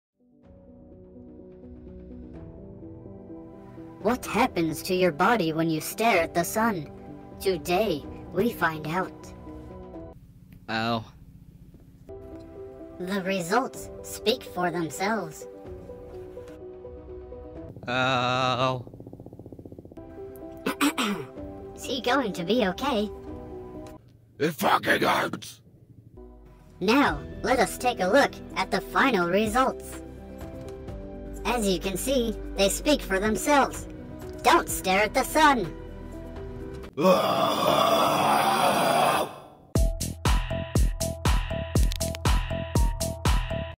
What happens when you Stare at the Sun?
In this video we will be breaking down the effects of staring at the sun. Possible health benefits!? Probably not.

This is satire making fun of the clickbait "health" videos that are all over YouTube nowadays.